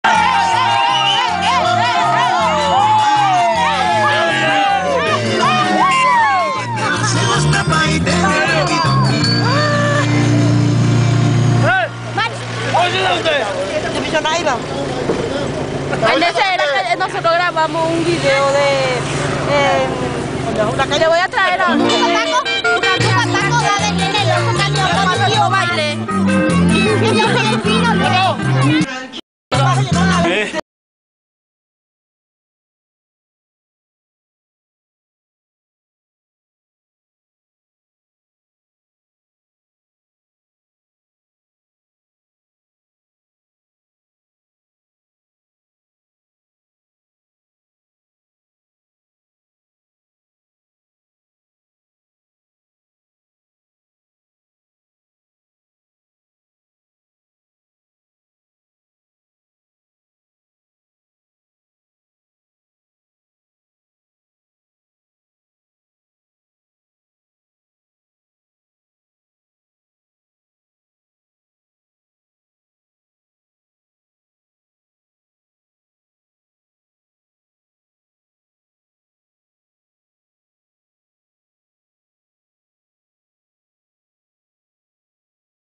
Celebrando el triunfo de Paso de Oro.MPG
Celebrating Paso de Oro's win on the way back home after the game.